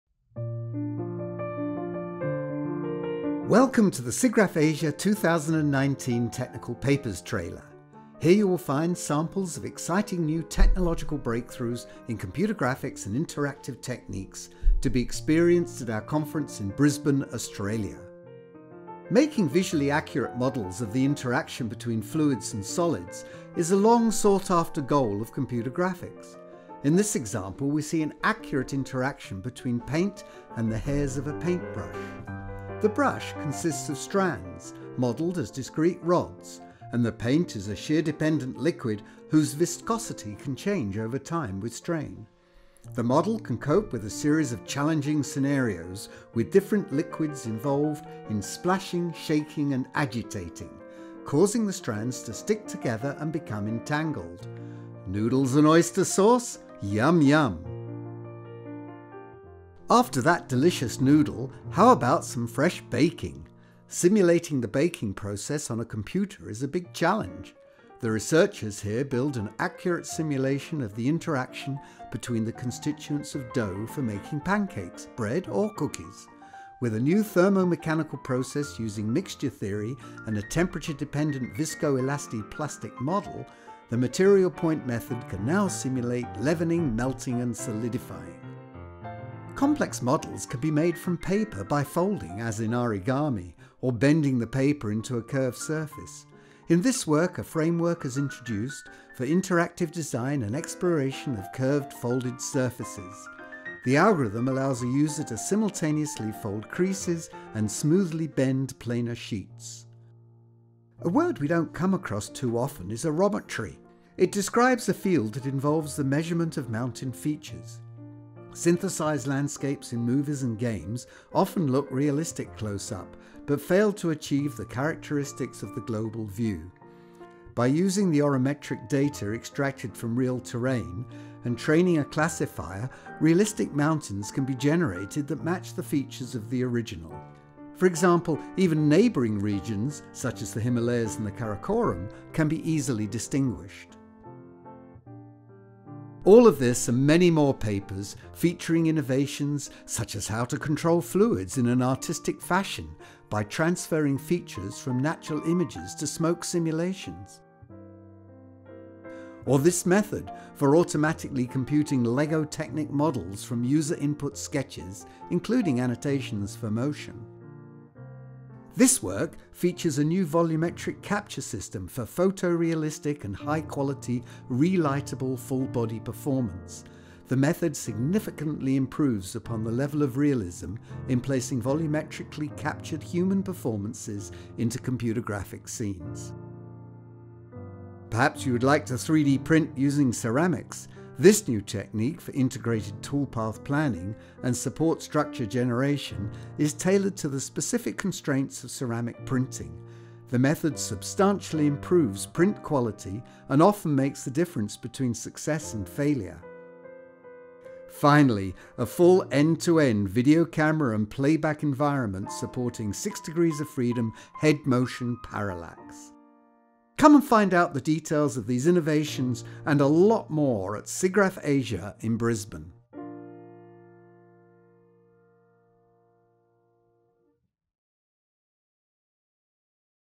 SIGGRAPH Asia 2019 – Technical Papers Trailer
Preview the SIGGRAPH Asia 2019 Technical Papers program!

The SIGGRAPH Asia Technical Papers program is the premier international forum for disseminating new scholarly work in computer graphics and interactive techniques. At the conference, paper authors provide brief overviews of their work in the Technical Papers Fast Forward event, then they present their complete papers in the sessions.

Technical Papers are published as a special issue of ACM Transactions on Graphics. In addition to papers selected by the SIGGRAPH Asia 2019 Technical Papers Jury, the conference presents papers that have been published in ACM Transactions on Graphics during the past year. Accepted papers adhere to the highest scientific standards.

Join us at the event to discover and learn more! to view the Technical Papers program.